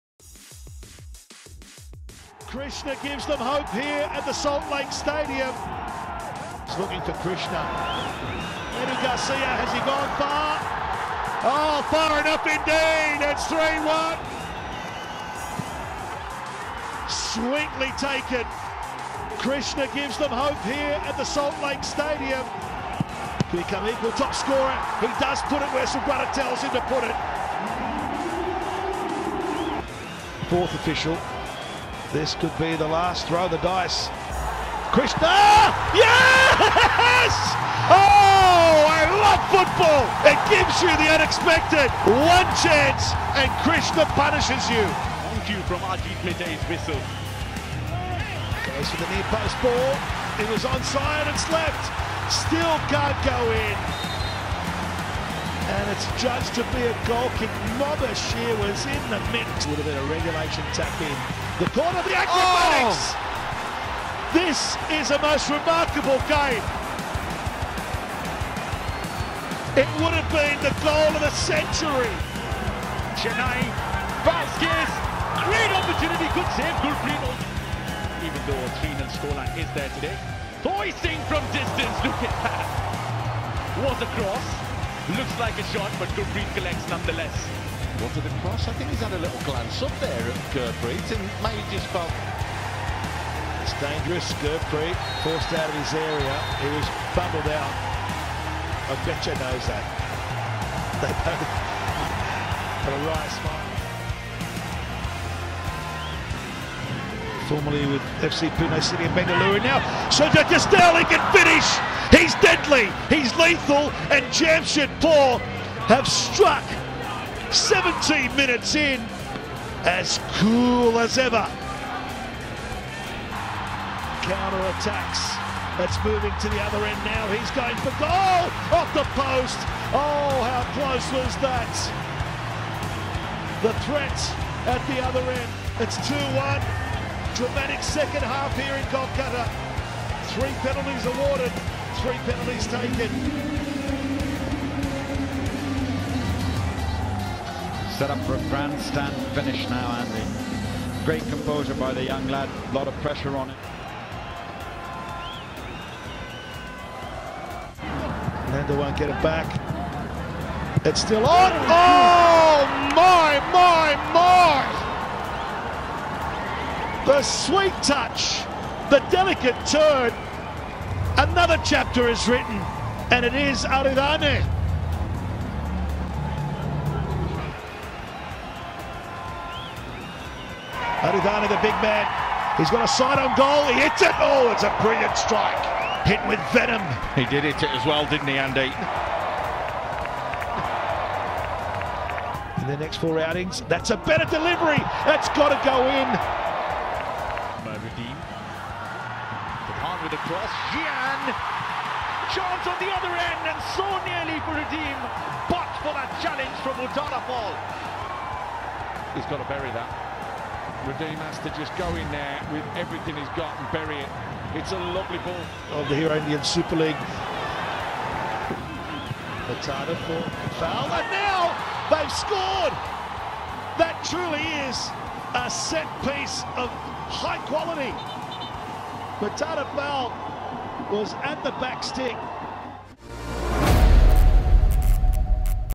Hero of the Month Nominees - November 2019 | Hero ISL 2019-20
Check out the Hero of the Month nominees for November 2019. Clinical goalscorers - Roy Krishna, Aridane Santana, Sergio Castel; a shot-stopper - Gurpreet Singh Sandhu & a robust defender - Mourtada Fall make it to the nominees for this month. Watch the video and make your pick.